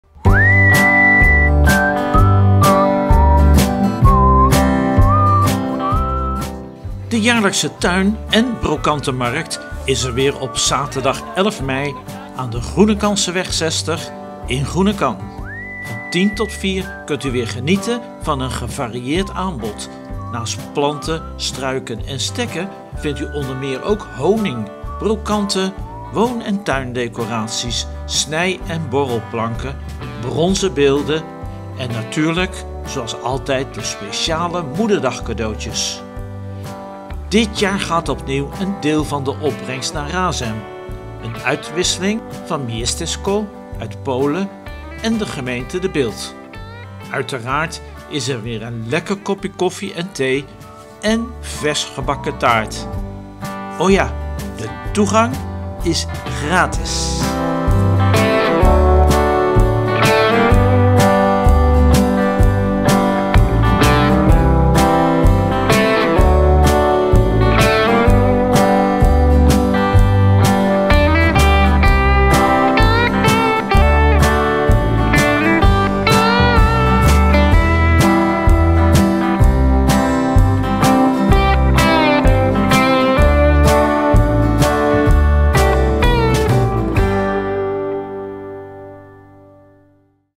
Tuin- en Brocantemarkt in Groenekan 11-5-2024
Op 11 mei is de jaarlijkse tuin- en brocante markt op de Groekanseweg 60 in Groenekan. Gratis entree